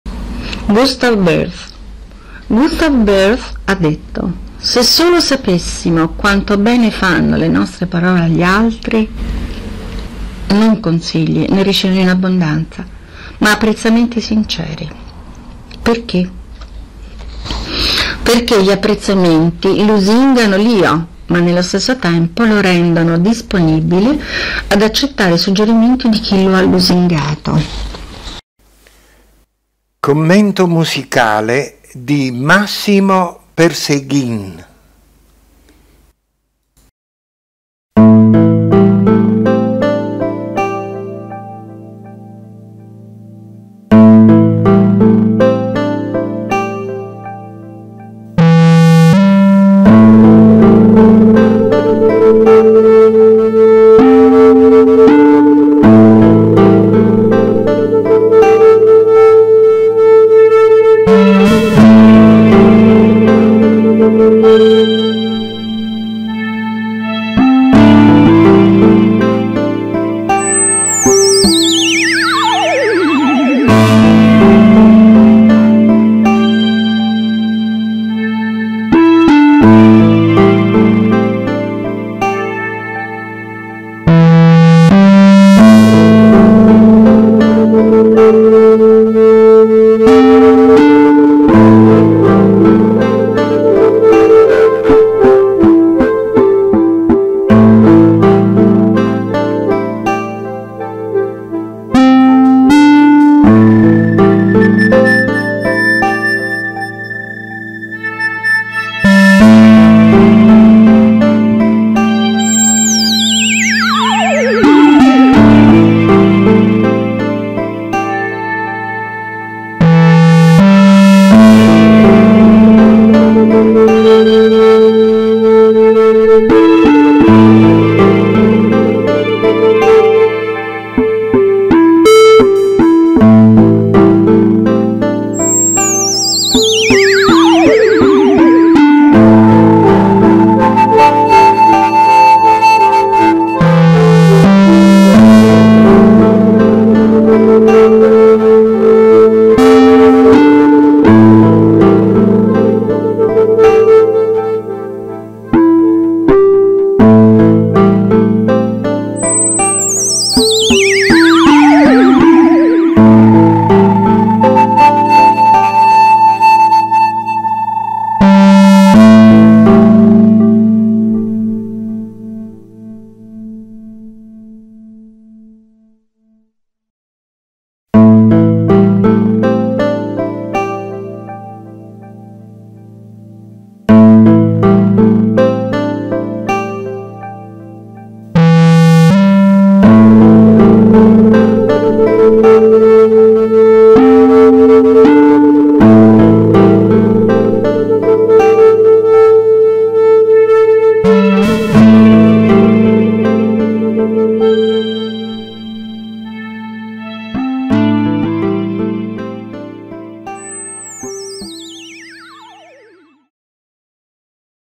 Evolvenza Gli apprezzamenti
- Gli apprezzamenti rendono disponibile l’uomo ad accettare i suggerimenti di chi lo lusinga.
- The appreciations make the man available to accept the suggestions of those who flatter him.
- Les appréciations rendent l'homme disponible pour accepter les suggestions de ceux qui le flattent.
- Las apreciaciones hacen que el hombre esté disponible para aceptar las sugerencias de quienes lo adulan.
- As apreciações tornam o homem disponível para aceitar as sugestões daqueles que o elogiam.
- Die Wertschätzung macht es dem Mann möglich, die Vorschläge derer anzunehmen, die ihm schmeicheln.
Evolvenza.it